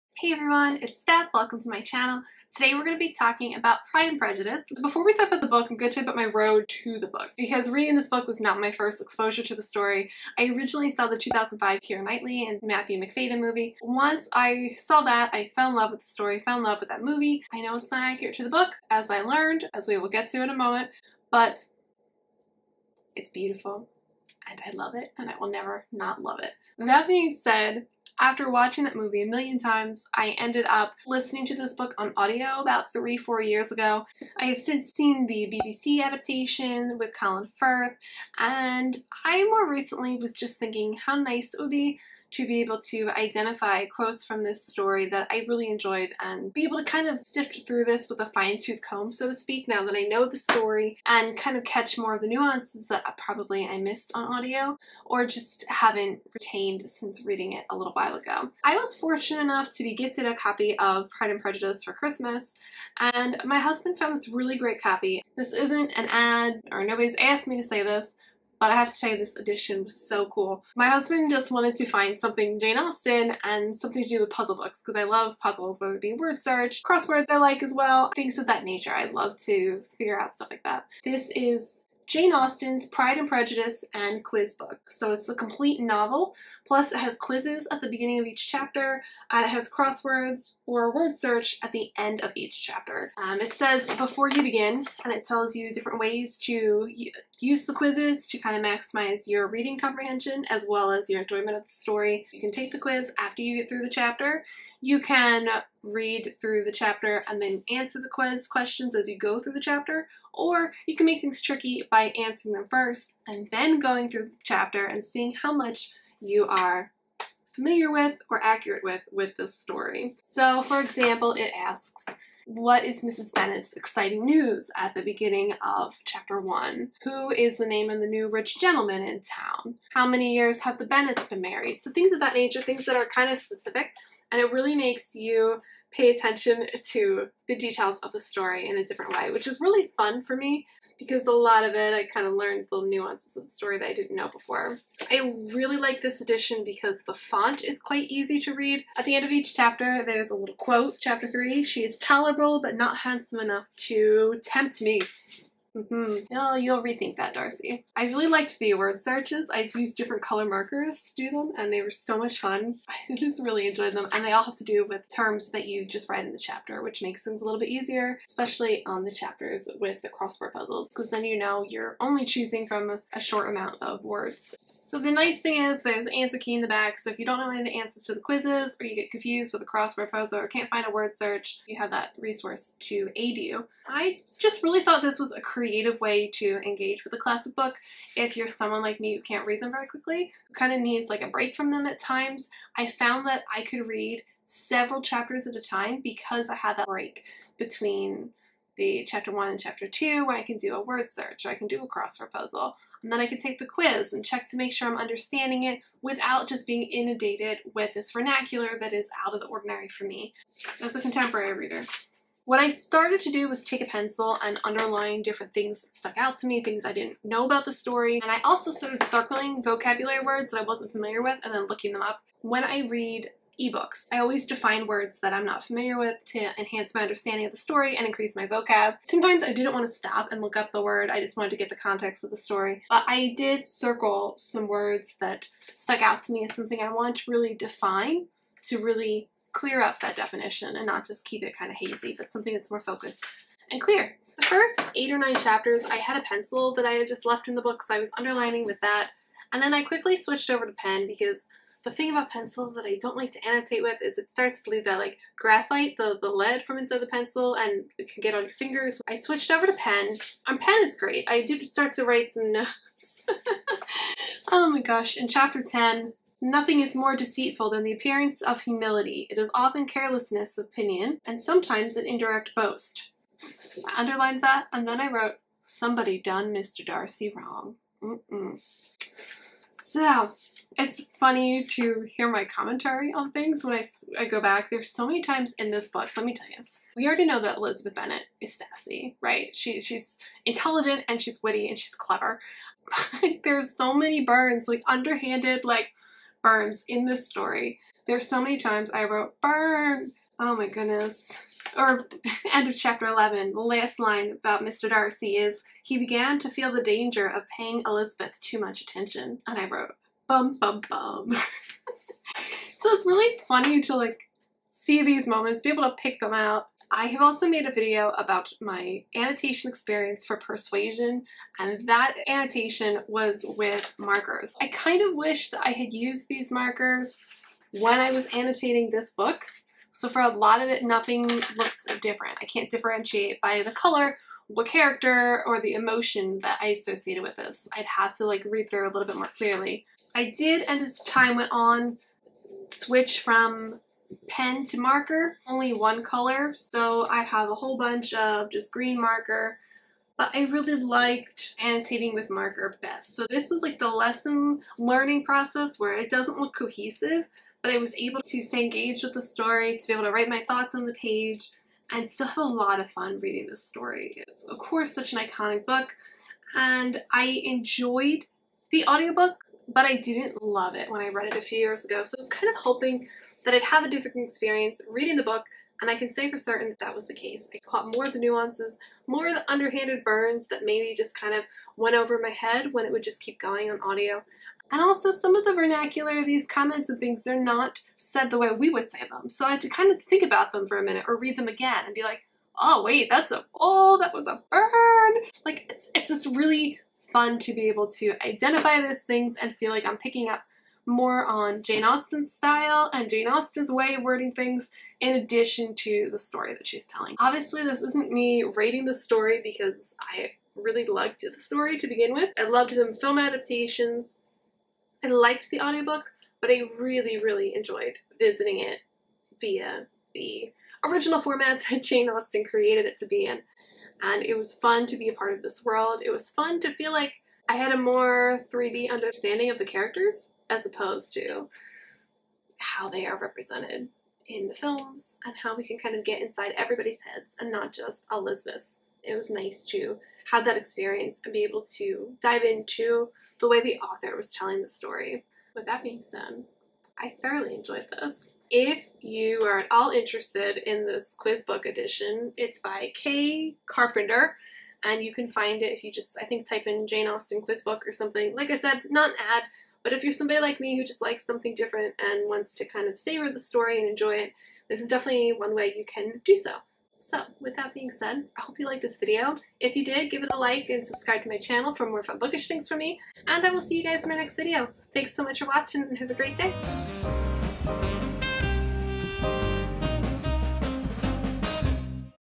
Annotating Jane Austen's Pride and Prejudice | Reading Jane Austen
Today’s topic is a discussion about how I annotated Jane Austen’s iconic novel, Pride and Prejudice. In this video we’ll discuss the strategies, resources and methodology I used to sift through this book and mark my favorite spots in the book. Whether you’re a Jane Austen newbie or a veteran, there’s something for everyone to enjoy in today’s video.